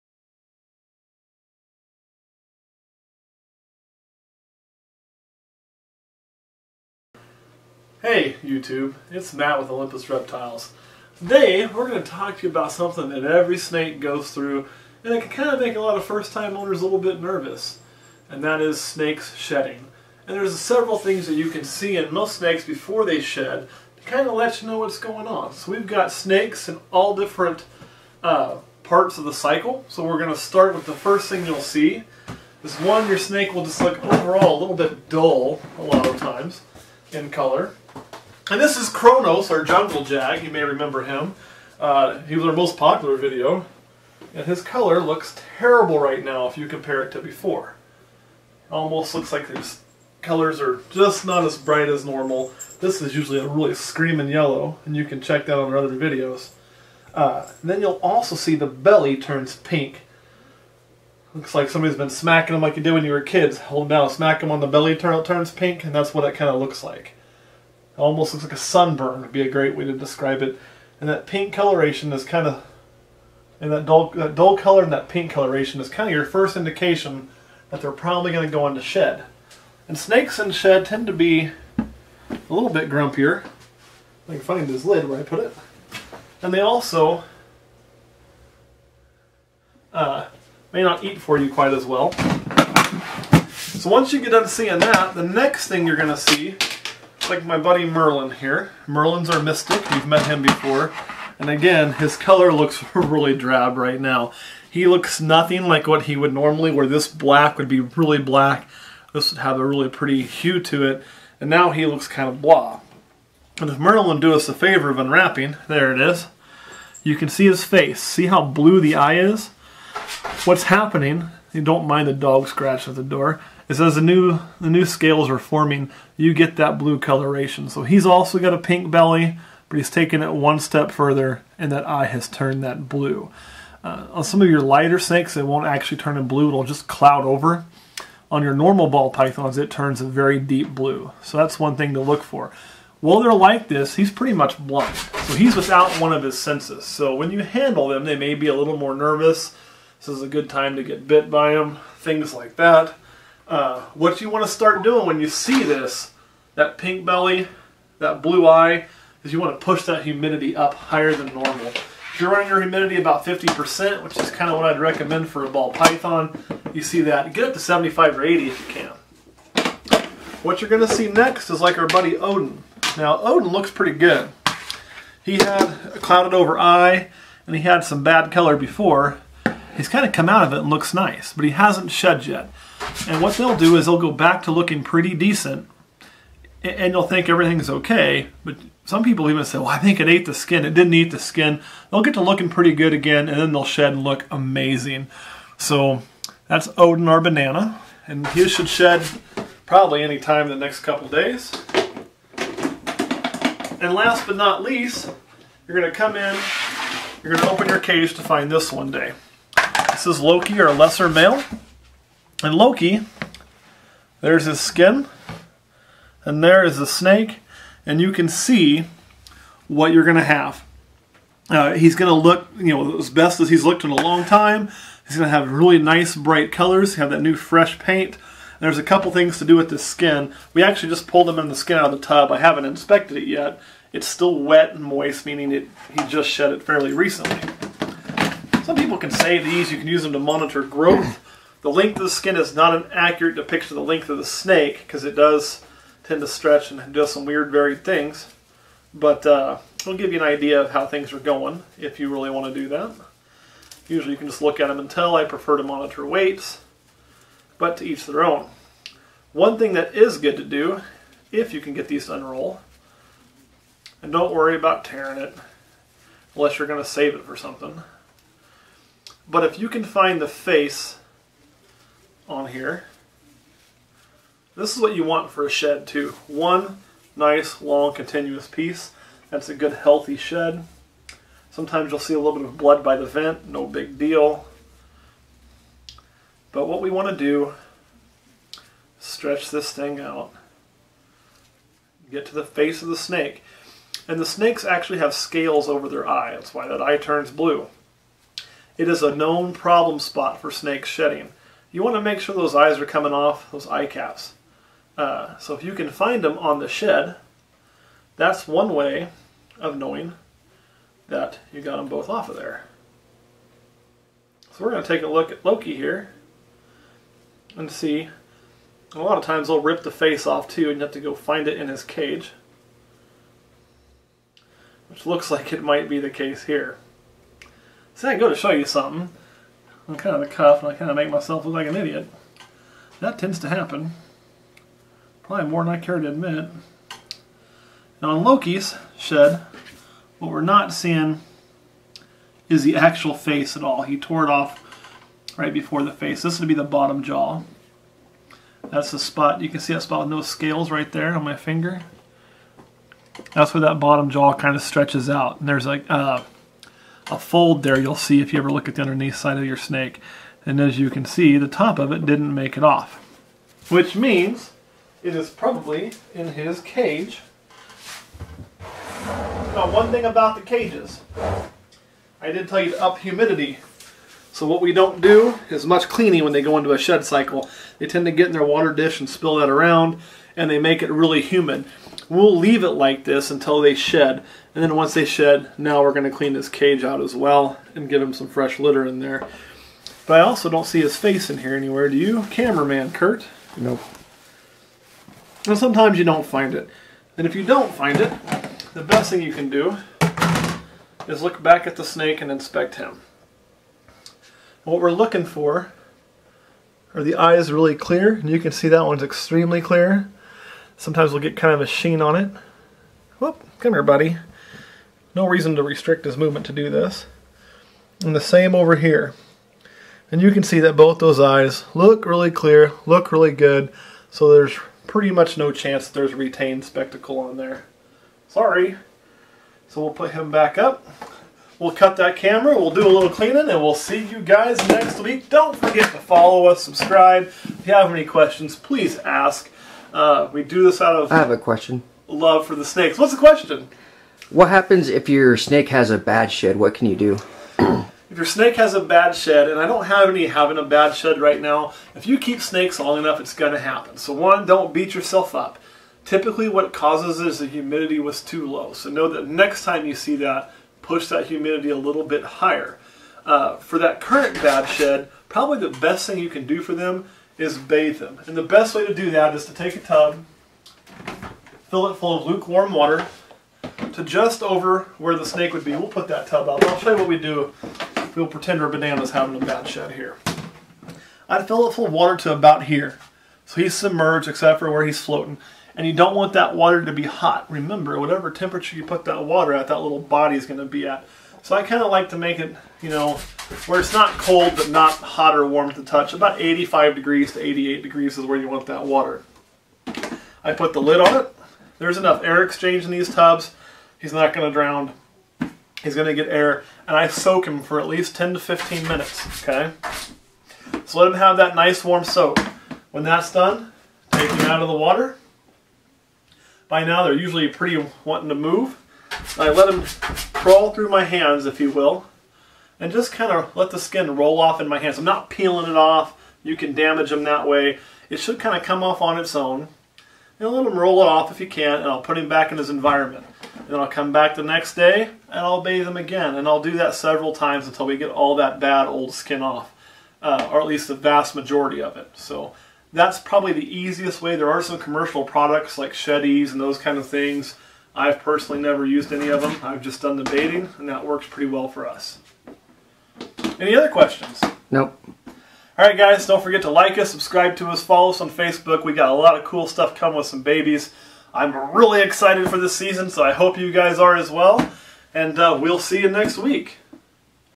Olympus Reptiles Episode #28 Snake Shedding
What to look for if you think your snake might be in shed.  How to tell if your snake is getting ready to start a shed cycle.